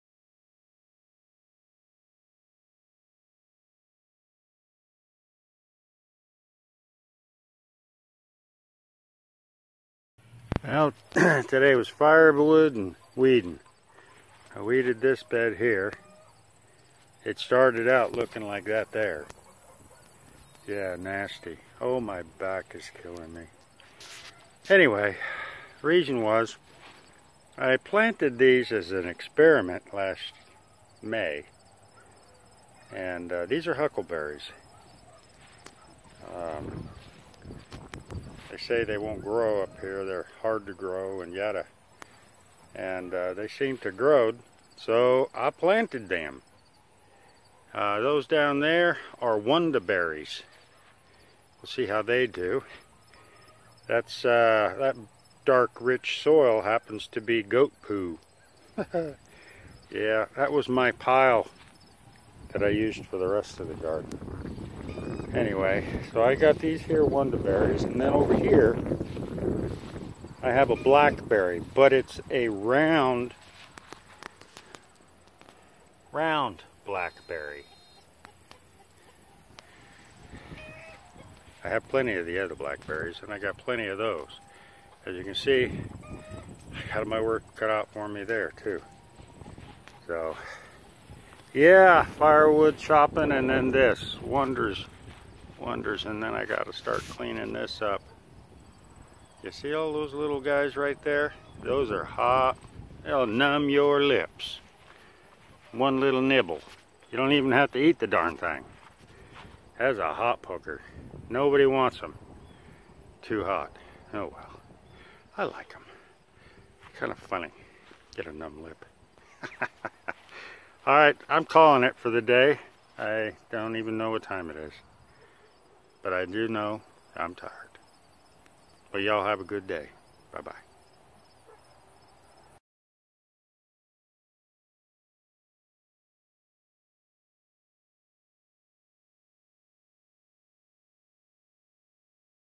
Weeding in the food forest.. a super shorty
Hey there, how you doing?
I said I was getting ready to put the garden to rest for the winter and here I am in the garden weeding and planting. I planted some Huckleberries, Wonderberries and  Round Blackberries. I was told that some of them won't grow where we are but I thought I would experiment some and guess what, I did some starts in May and  they grew so I thought it would be a great time to put them in the ground.

Mr Country Ken